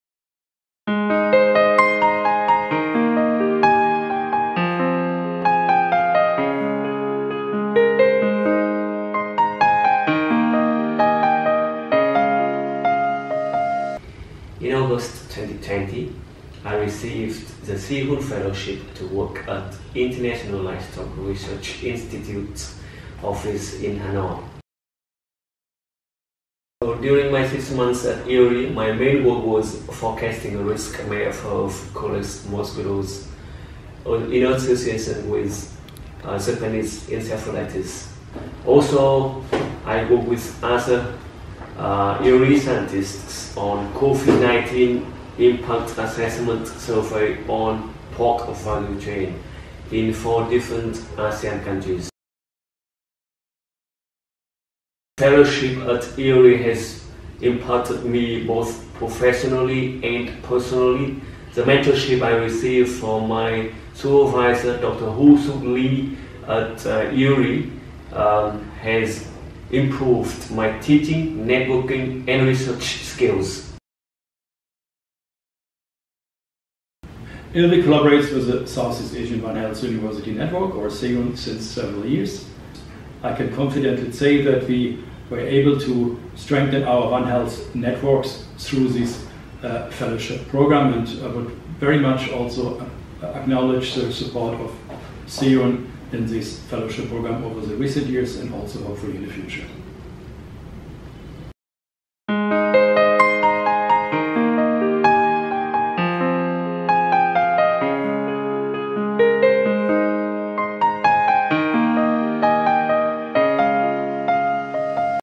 Developing the capacity of young One Health professionals in Southeast Asia
Young professional from Southeast Asia have benefitted from capacity development in One Health under the ILRI and Southeast Asia One Health University Network (SEAHOUN) collaboration. Some of the professionals share their experiences from the program in this film. The fellowship is funded by the United States Agency for International Development (USAID) through the One Health Workforce-Next Generation project.